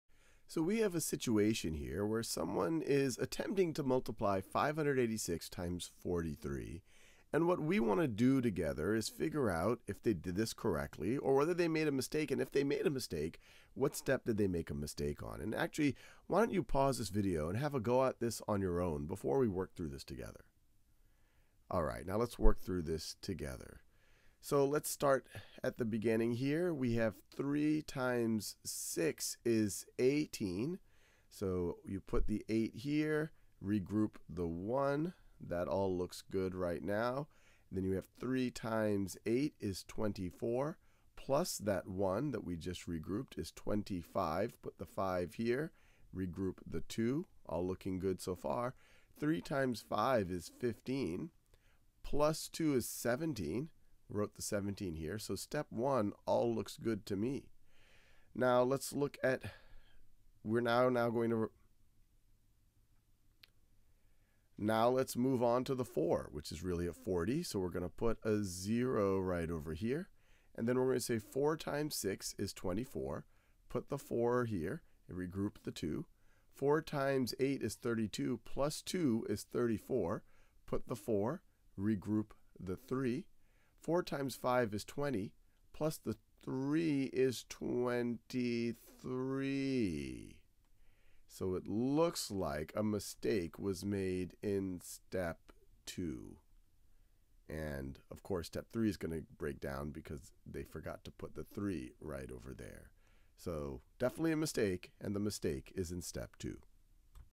Multiplying 3-digit by 2-digit numbers: Error analysis | Grade 5 (TX TEKS) | Khan Academy
Find the error while multiplying 3-digit by 2-digit numbers by checking each step.